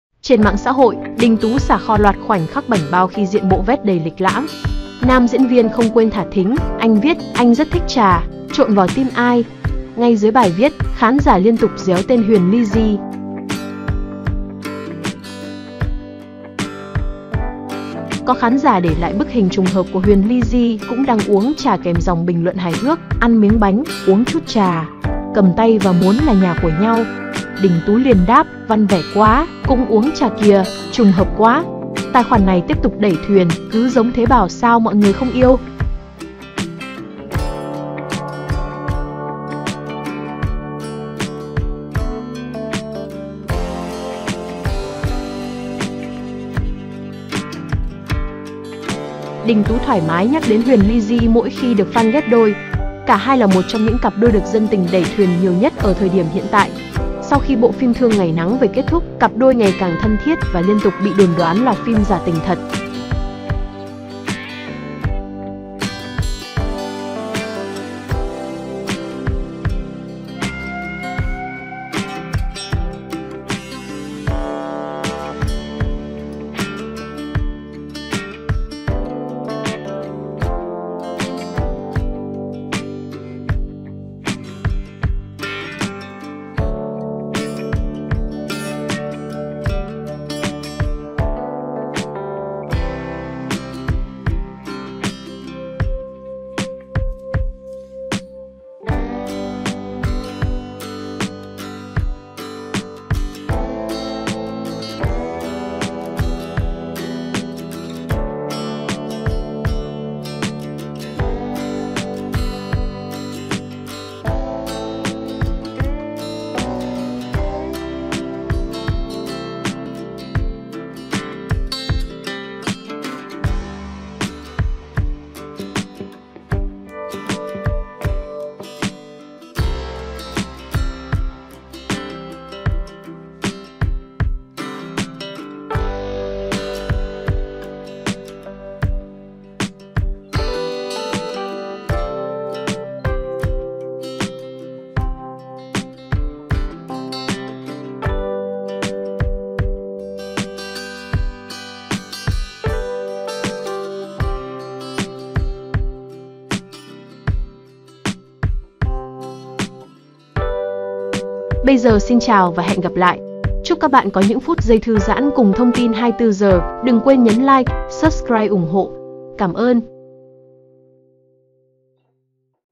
Đăng ảnh "thả thính", netizen tìm ra điểm chung giữa Huyền Lizzie và Đình Tú.
Đình Tú thoải mái nhắc đến Huyền Lizzie mỗi khi được fan ghép đôi.